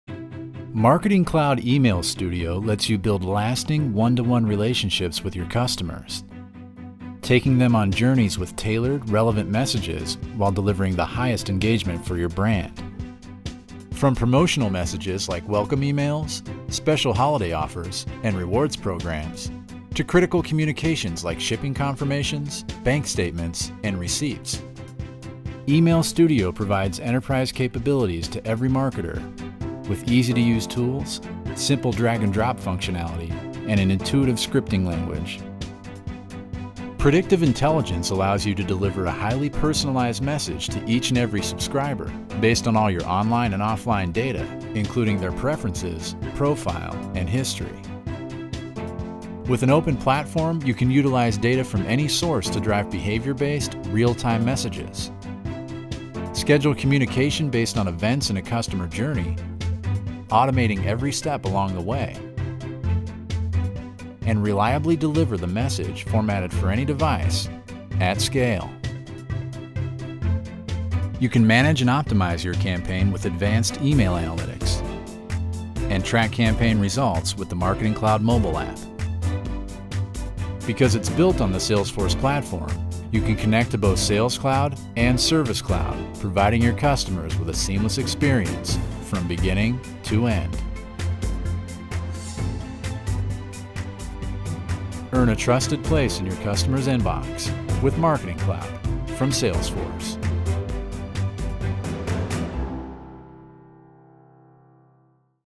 Salesforce Marketing Cloud: Email Studio Demo
Learn more about creating highly personalized email campaigns with industry-leading scalability and ease.